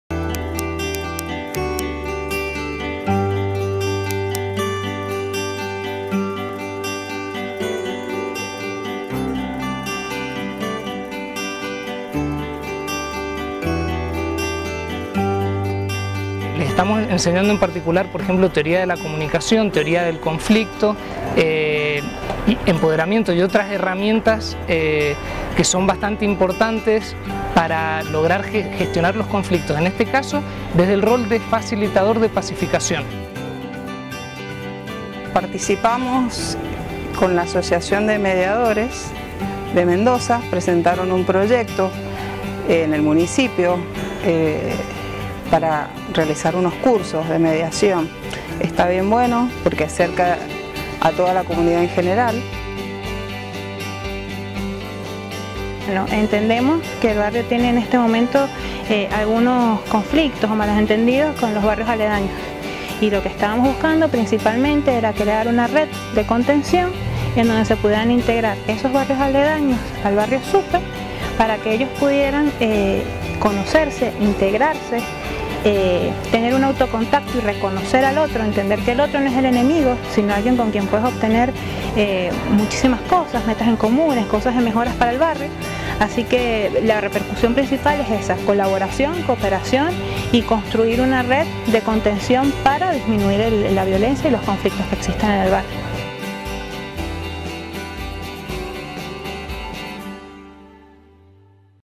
Programa +Comunidad: Proyecto Facilitadores de Pacificación
Los vecinos del barrio SUPE de Godoy Cruz presentaron este proyecto y fueron seleccionados para llevar adelante su ejecución.